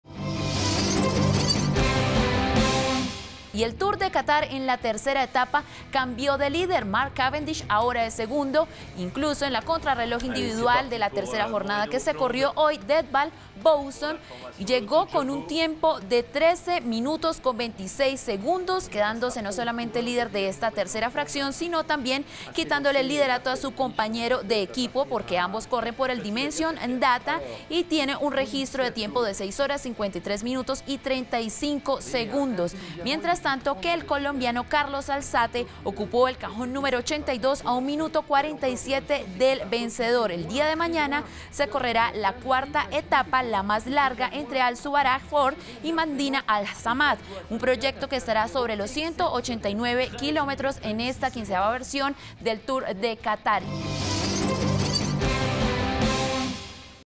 Edvald Boasson superó a Mark Cavendish en la CRI de la Vuelta a Qatar
Video Claro Sports Colombia